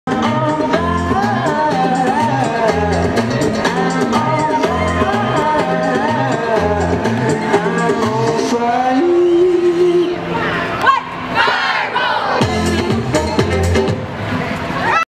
Cheerleaders perform during parade
The Ridgway Area cheerleaders performed Saturday, Aug. 23, 2016 during the annual Ridgway Firemen's Carnival Parade.